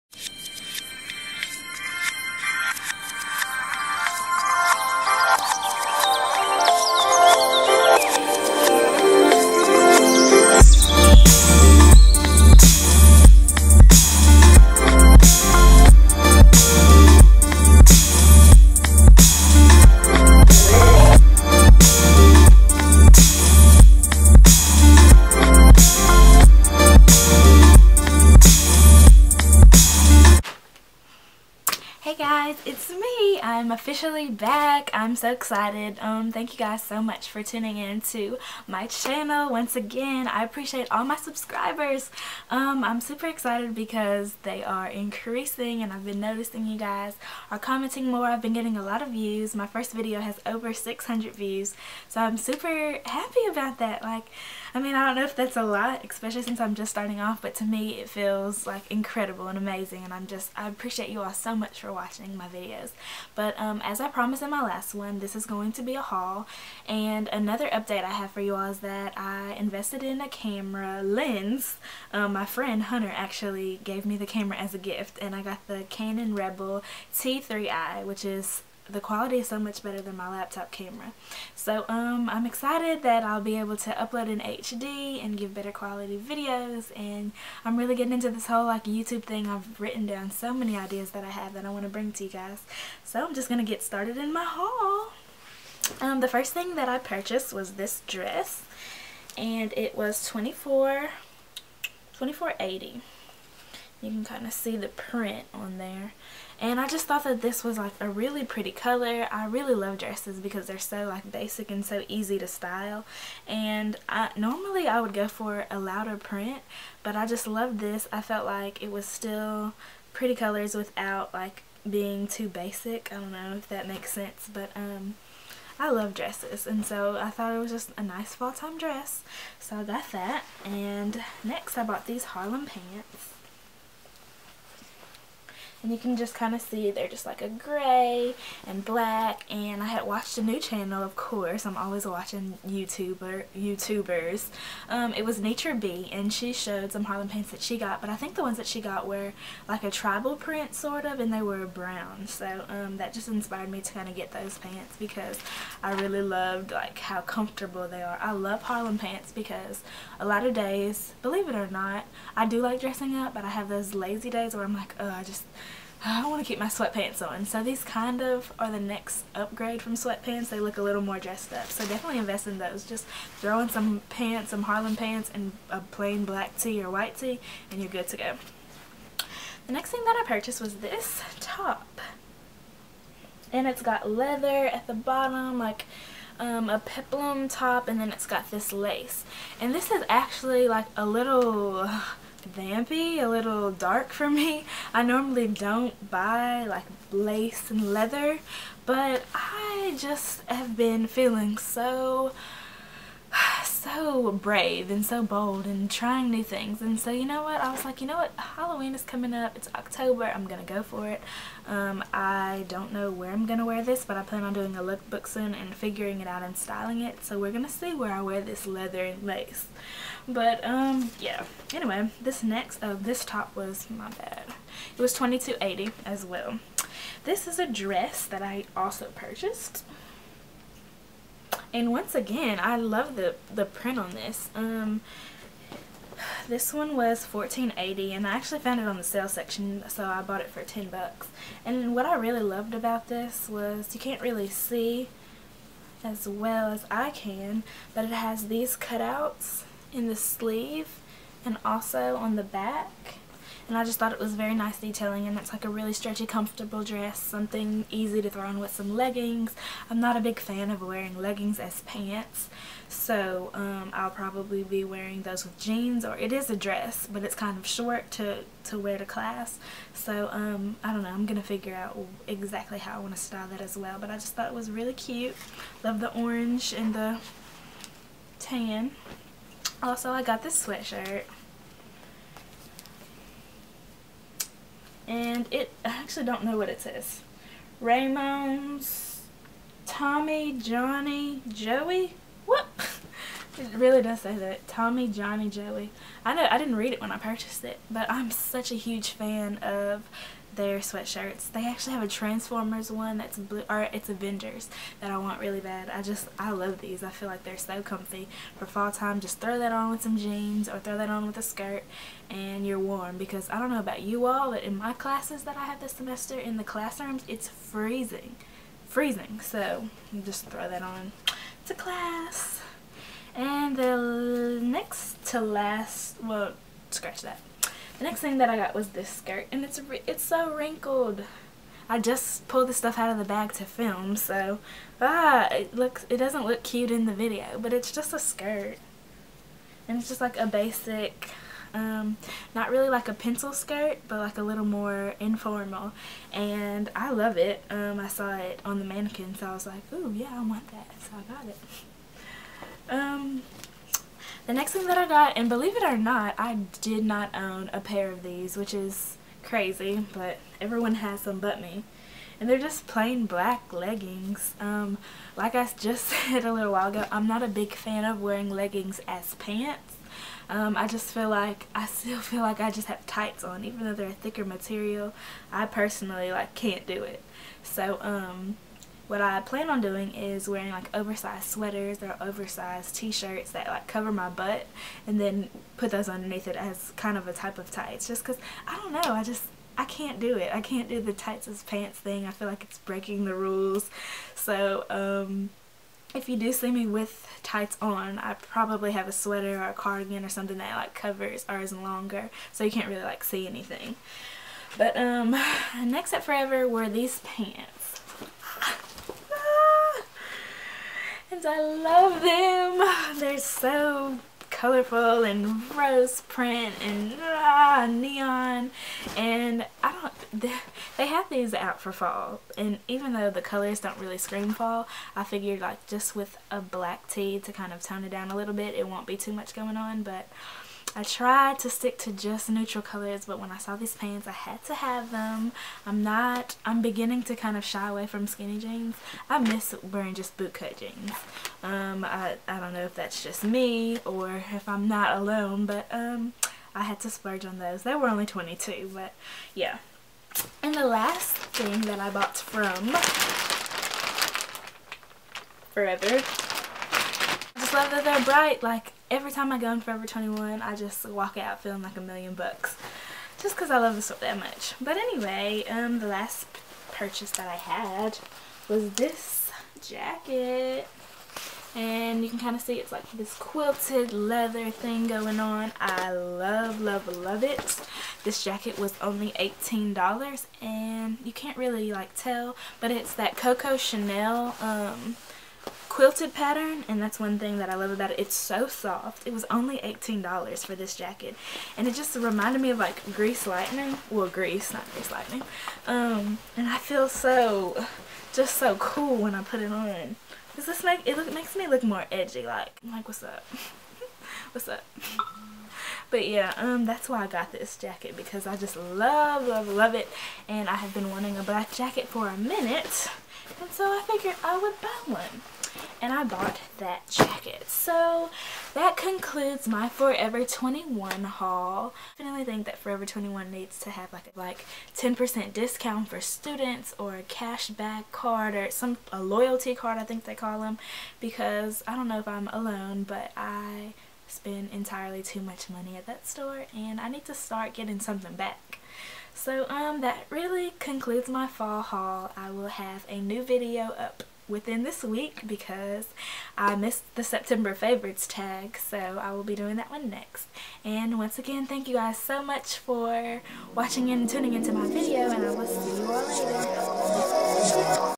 Forever Fall Fashion Haul | TheFashionBank ♡
Dress: Forever 21 (Sale Section)
Necklace: Dillards
Headband: Forever 21
Camera: Canon Rebel T3i
Edit: iMovie 09